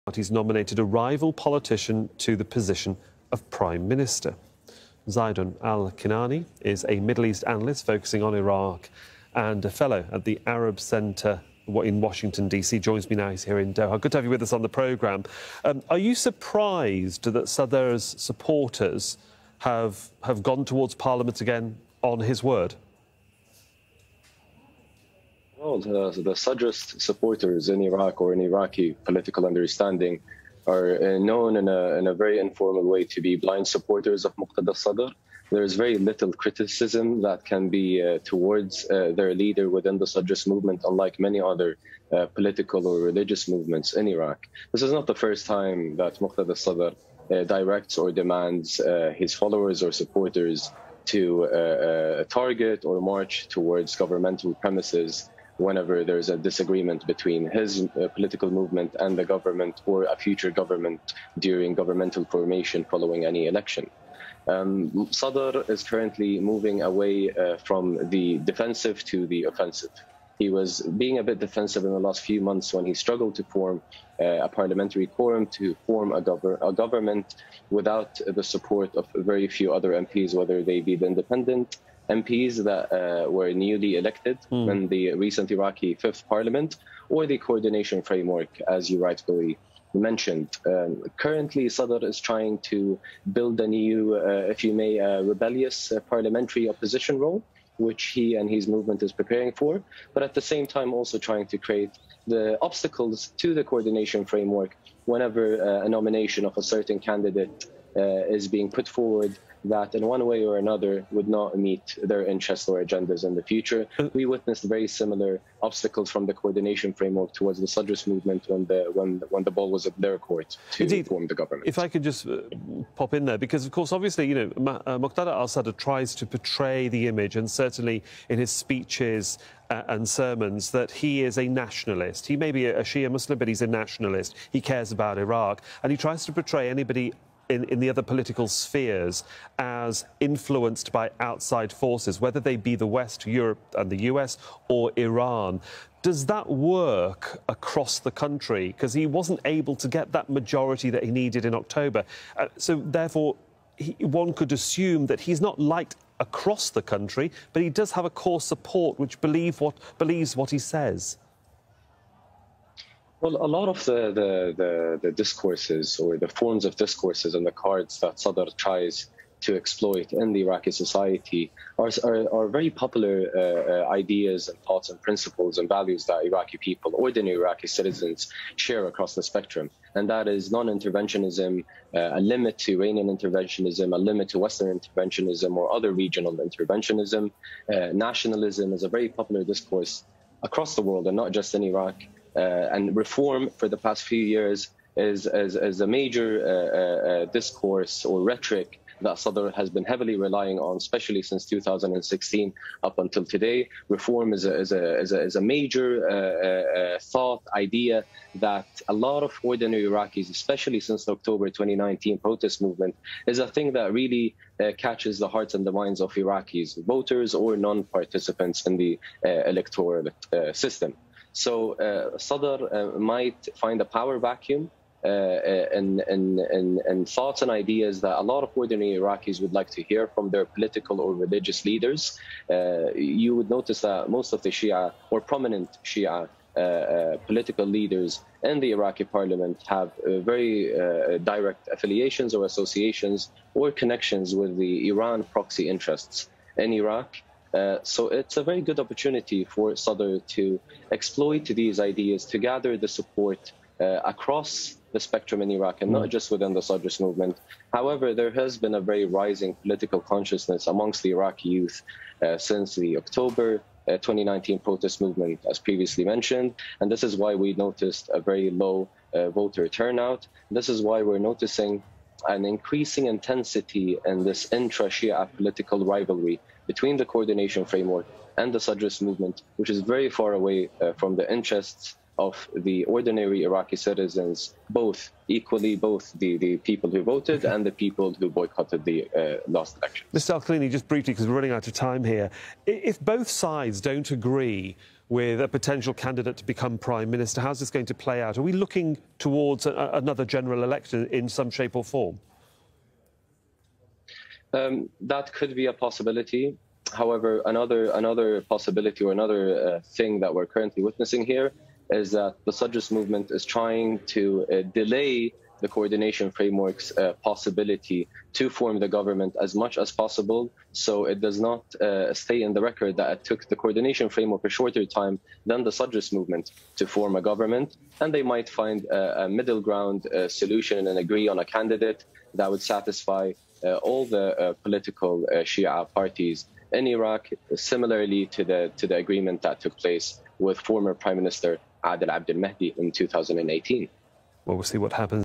Followers of Sadrist movement storm Iraqi parliament - Zeidon Alkinani - Aljazeera English
Zeidon Alkinani, Iraq-focused Middle East political analyst explains the motives behind the storming of Iraqi parliament by the followers of the Iraqi Shia cleric and political leader Muqtada Al-Sadr for the second time in one week.